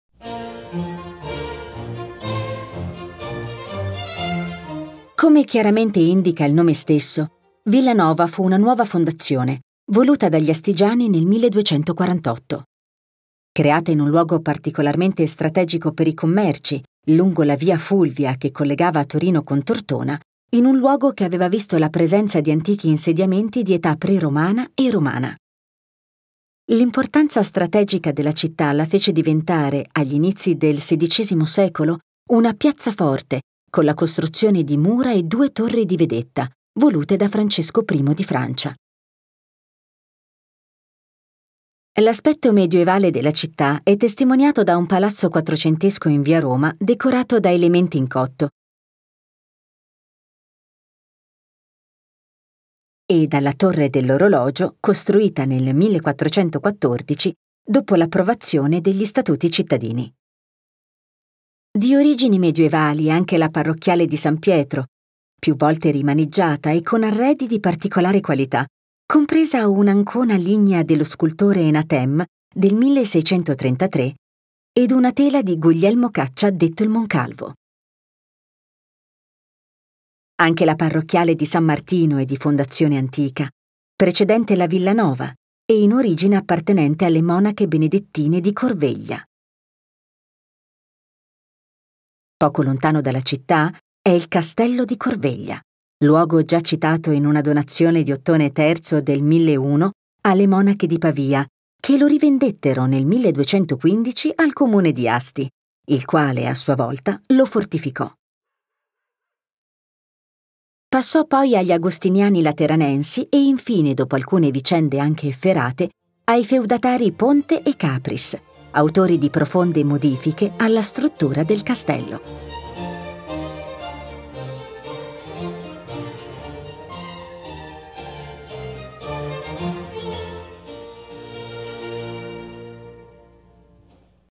Videoguide - Villanova d'Asti: arte e storia
Città fondata dagli astigiani nel 1248 sull’antica via Fulvia, Villanova presenta vari punti d’interesse per il visitatore come la torre dell’orologio, la parrocchiale di san Pietro e il borgo Corveglia posto poco fuori dall’abitato.